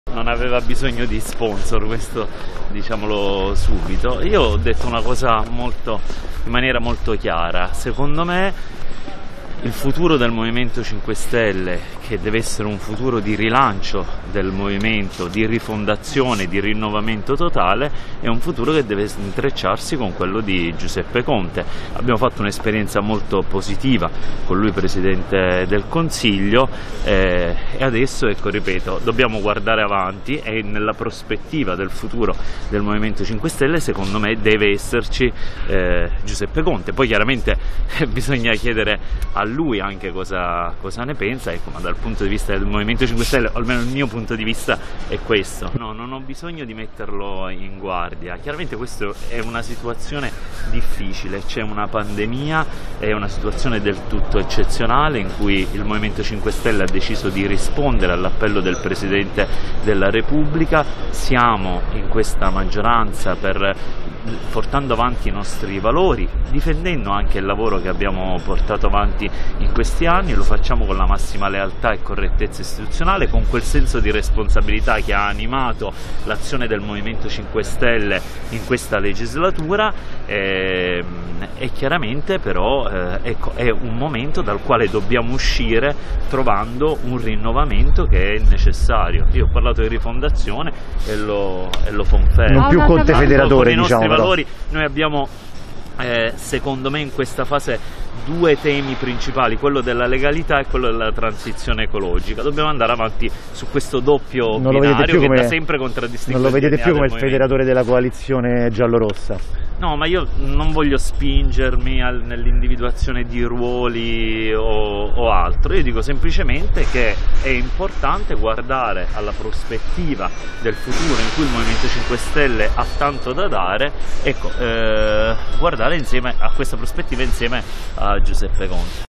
M5s, Bonafede: «Conte? Futuro del Movimento 5 stelle passa da lui»
L'ex ministro della Giustizia fuori da Montecitorio

\"Conte? Secondo me il futuro del Movimento, che deve rifondarsi, è un futuro che deve intrecciarsi con il futuro di Conte. Con lui esperienza positiva di Governo. Dobbiamo guardare avanti e nel futuro deve esserci Conte\", così Alfonso Bonafede, ex ministro della Giustizia e deputato del Movimento 5 Stelle. \"Metterlo in guardia? Non c'è bisogno. È una situazione eccezionale. Abbiamo risposto all'appello di Mattarella portando avanti i nostri valori. È un momento da cui dobbiamo uscire rinnovandoci. In questo frangente abbiamo due temi: legalità e transizione green\", ha spiegato Bonafede. ( Ansa - CorriereTv ). Guarda il video su Corriere